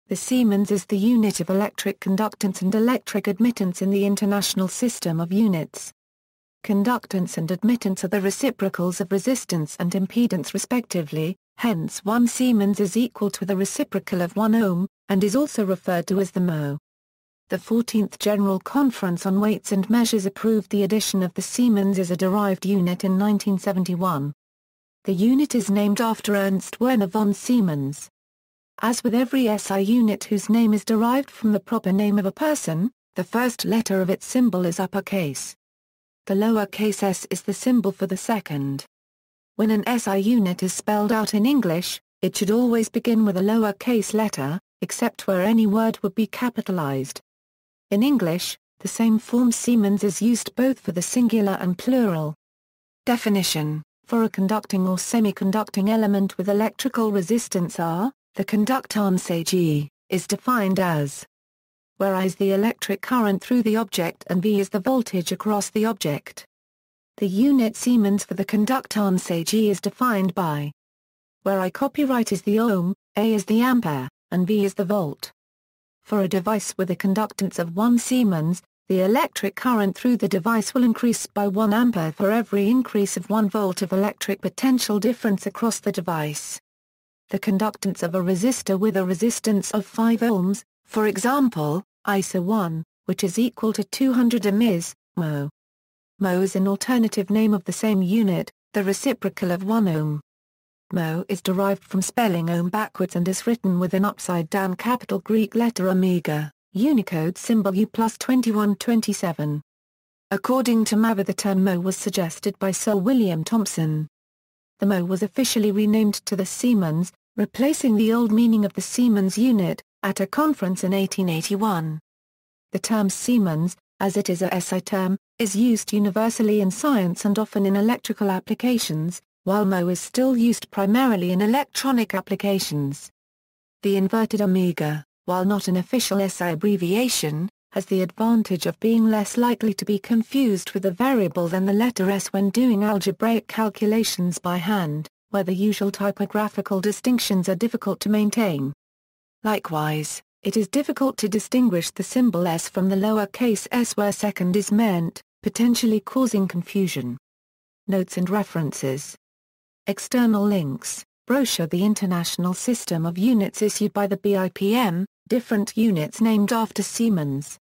Siemens (unit)
The siemens (SI unit symbol: S) is the unit of electric conductance and electric admittance in the International System of Units (SI). Conductance and admittance are the reciprocals of resistance and impedance respectively, hence one siemens is equal to the reciprocal of one ohm, and is also referred to as the mho. The 14th General Conference on Weights and Measures approved the addition of the siemens as a derived unit in 1971.
The unit is named after Ernst Werner von Siemens. As with every SI unit whose name is derived from the proper name of a person, the first letter of its symbol is upper case (S); the lower-case "s" is the symbol for the second. When an SI unit is spelled out in English, it should always begin with a lower-case letter (siemens), except where any word would be capitalized. In English, the same form siemens is used both for the singular and plural.